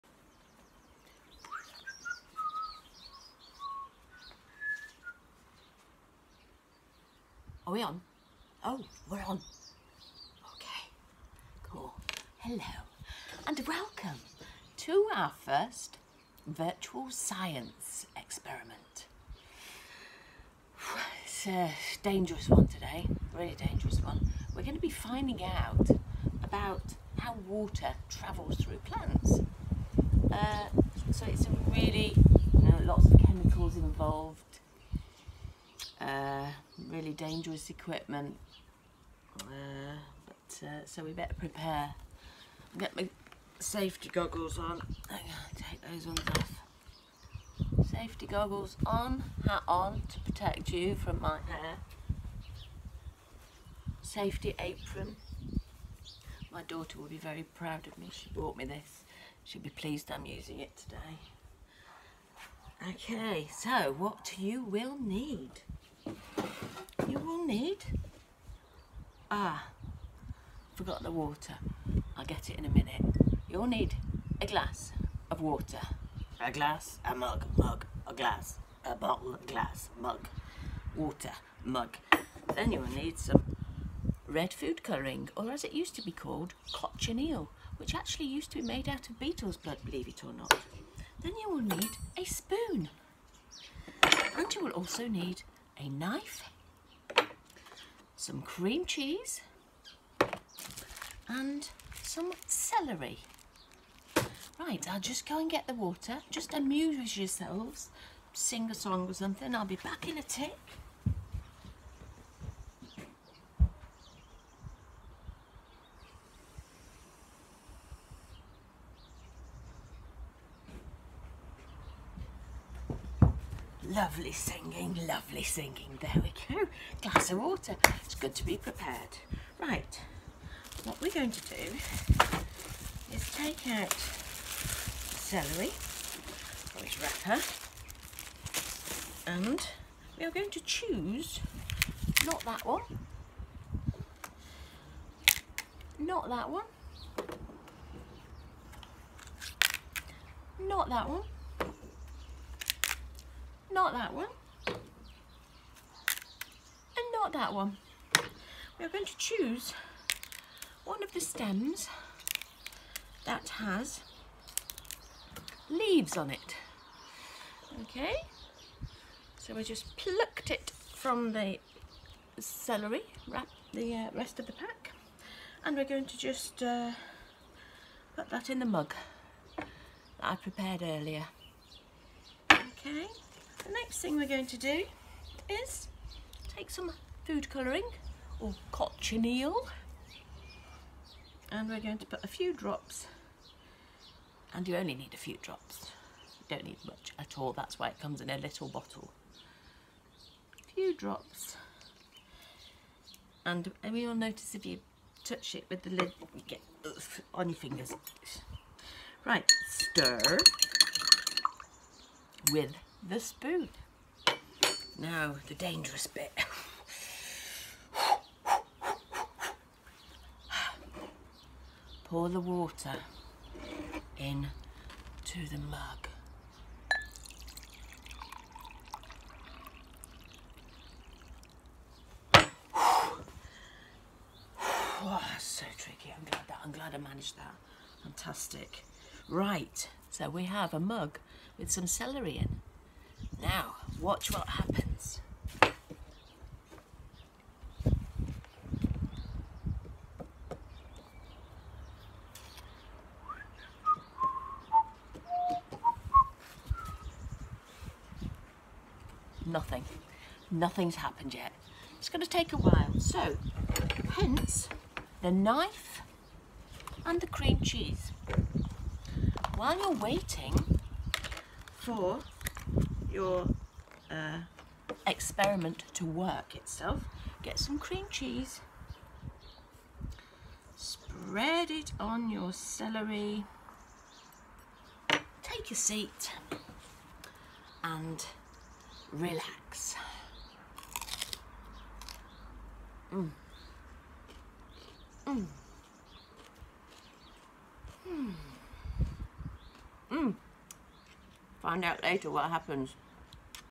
Year 3 lesson on capillary action part 1
A virtual lesson on how water travels through a plant.